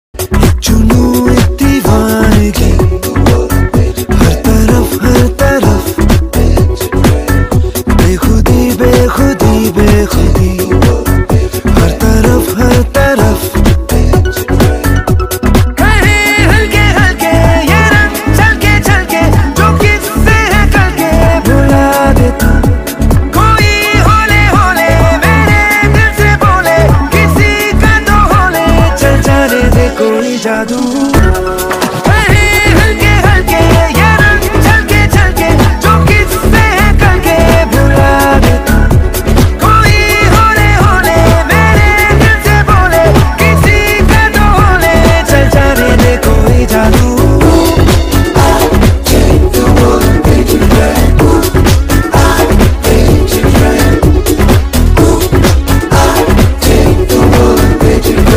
painting Competition
This video I have taken in in my school campus on the occasion of on the spot painting competition. this video especially for the student who is learning the art and craft.